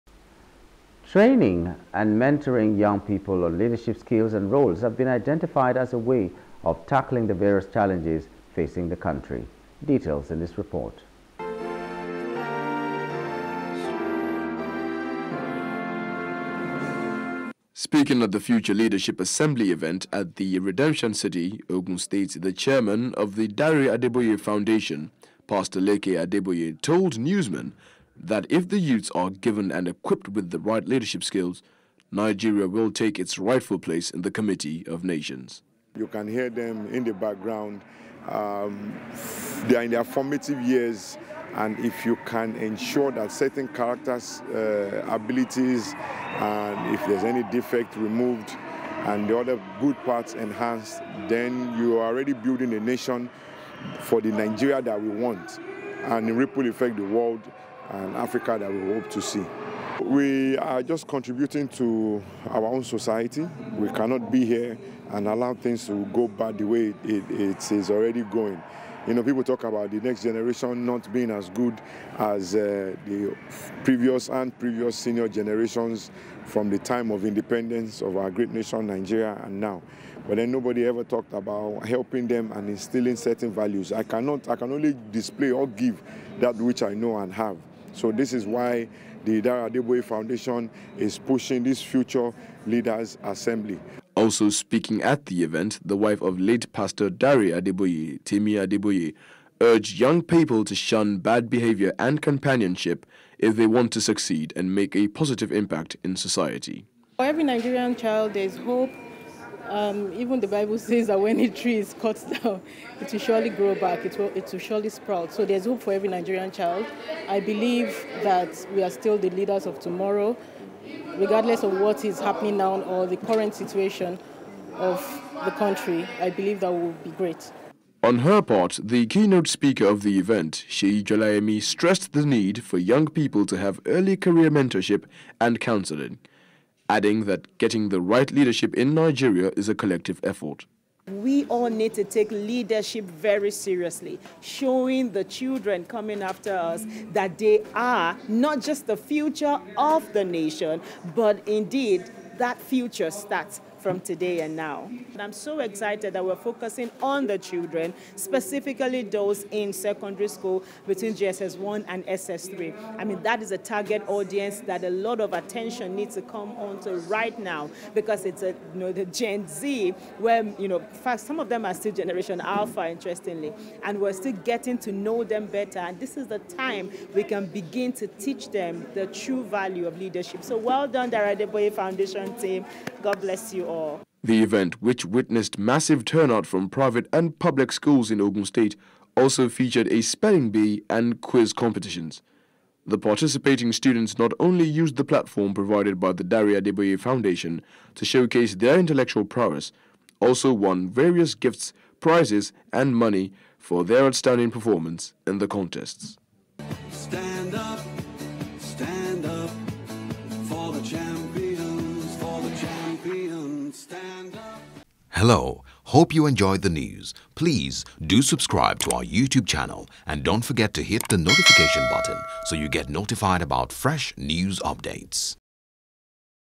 Future Leadership Assembly Mentoring Youths On Leadership Key To Tackling Nat'l Challenges-Adeboye
Training and mentoring young people on leadership skills and roles have been identified as a way of tackling the country's various challenges.